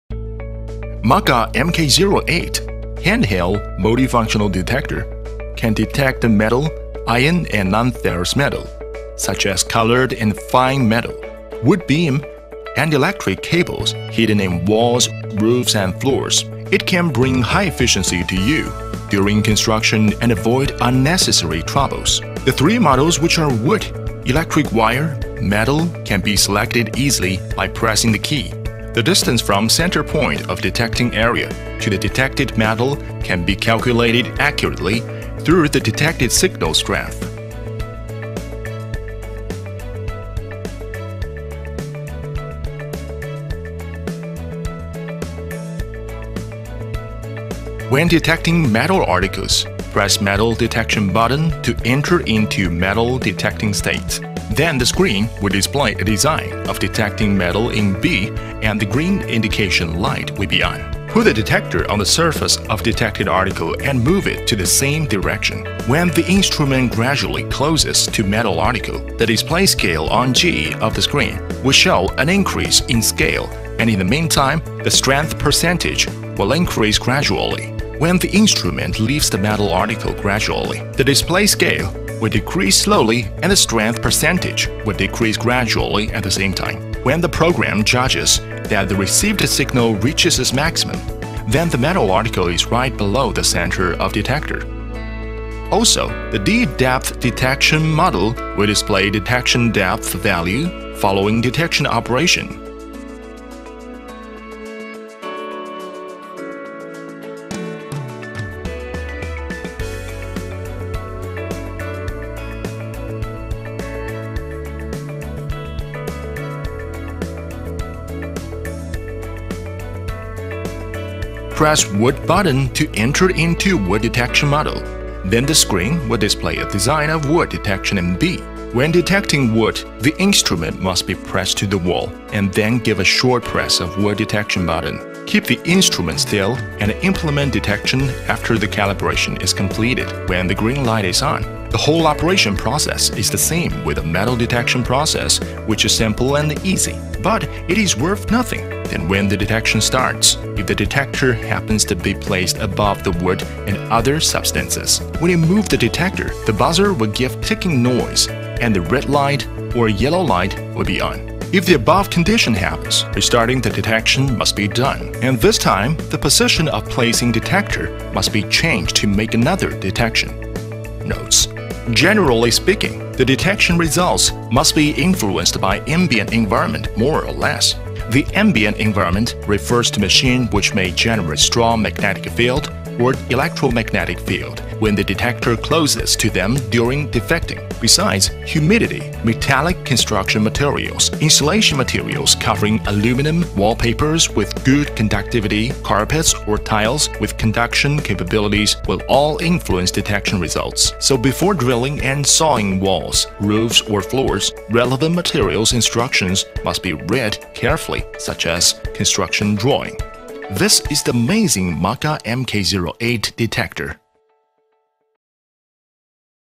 MK08 Digital Wall Detector
Product Detail:
Maximum scanning depth:
Ferrous metals: 80mm
Wood: 20mm
Copper conductors(live**): 50mm
Automatic switch off after approx: 5min
Operating temperatore: -10℃+40℃
Storage temperature: -20℃+60℃
Battery: 1x9V
Battery lasts: 6h
Product size:146*67.5*27mm